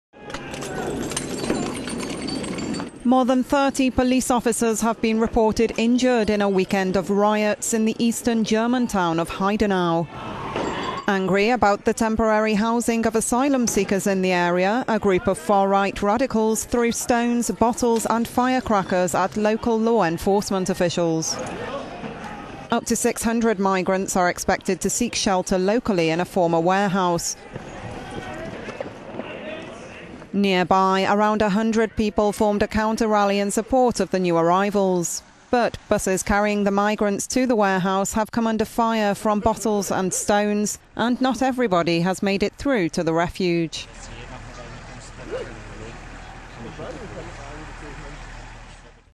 Germany: Heidenau police officers injured in weekend of anti-migrant riots
More than 30 police officers have been reported injured in a weekend of riots in the eastern German town of Heidenau.

Angry about the temporary housing of asylum seekers in the area, a group of far-right radicals threw stones, bottles and firecrackers at local law enforcement officials.

Up to 600 migrants are expected to seek shelter locally in a former warehouse.

Nearby, around a hundred people formed a counter-rally in support of the new arrivals.